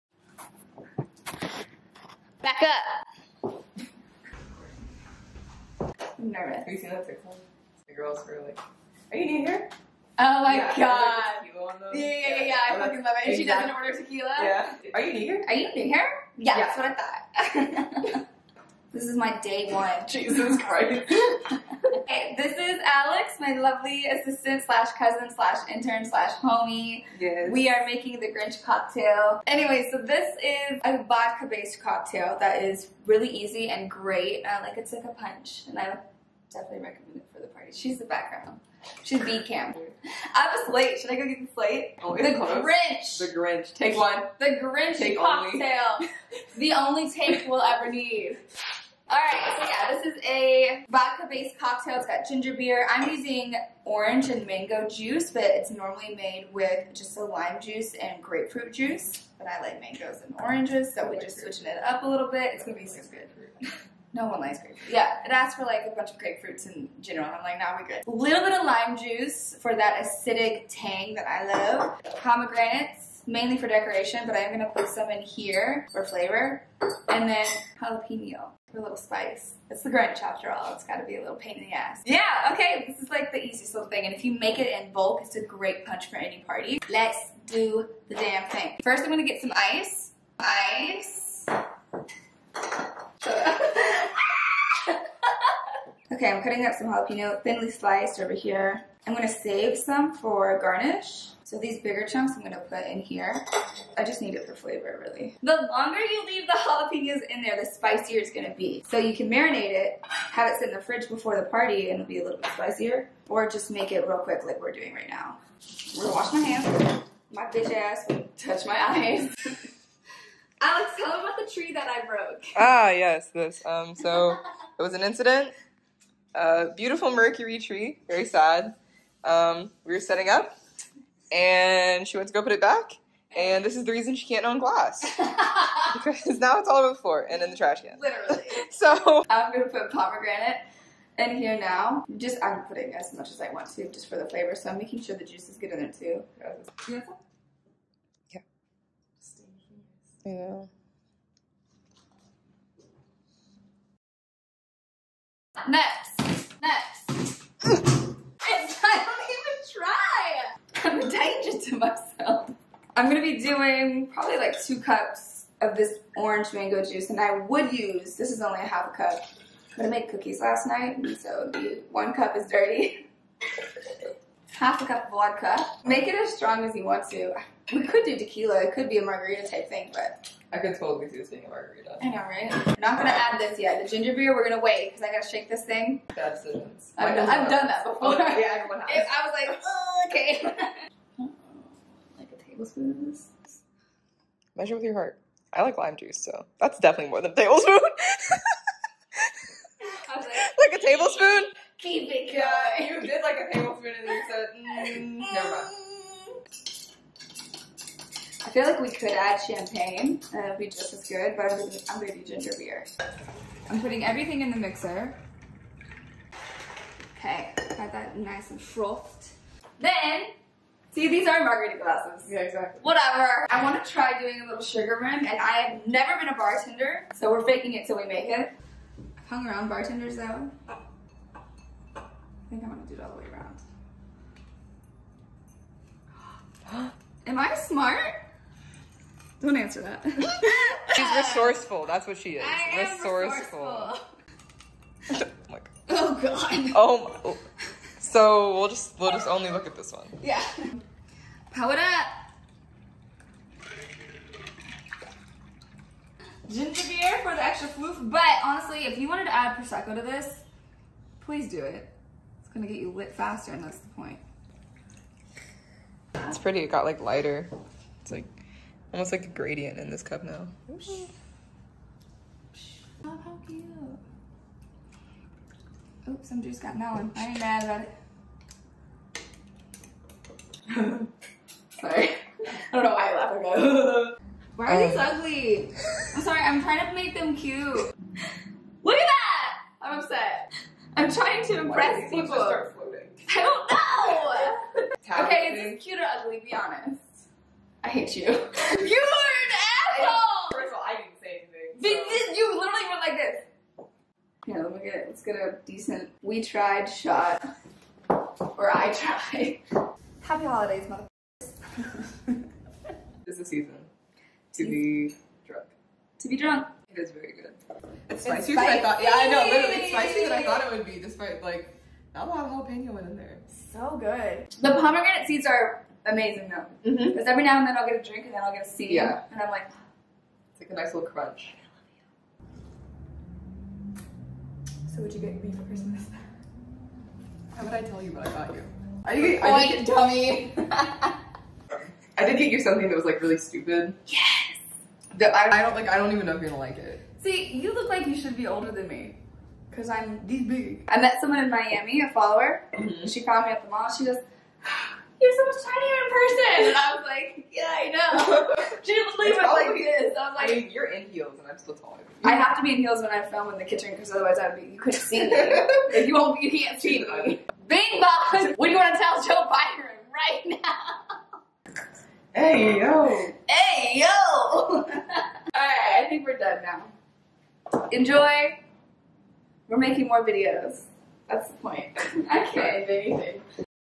THE GRINCH COCKTAIL
Make a cocktail with us!
This is a vodka based cocktail with a spicy kick, but I welcome you to use tequila and/or Prosecco for your version. :P

Faux Leather Pants (wearing a 2)
T-Shirt (wearing a small)

Martini Shaker